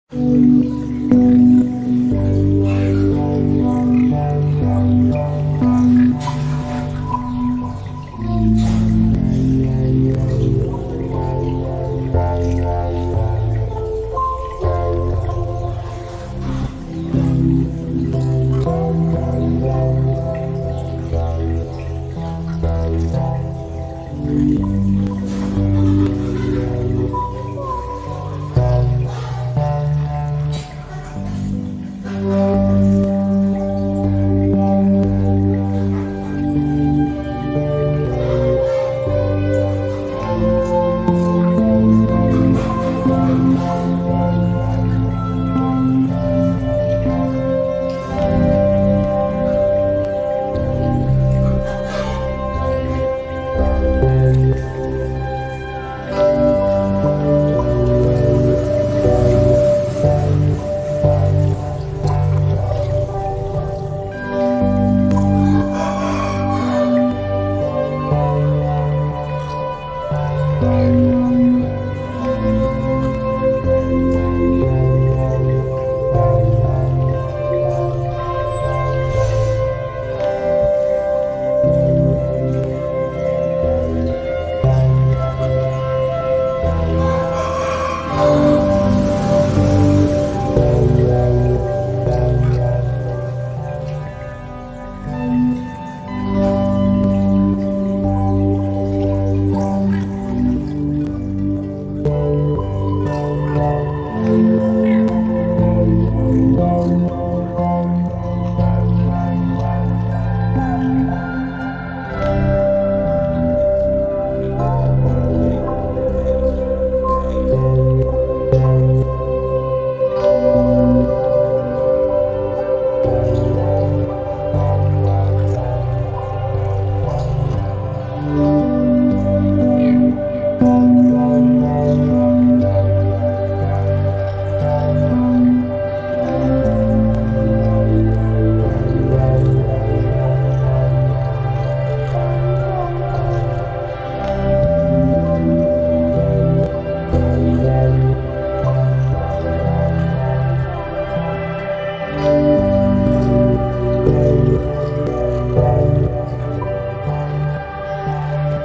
Aquarium fish travel underwater.
aquarium fish travel and they have show many different fish are together in underwater.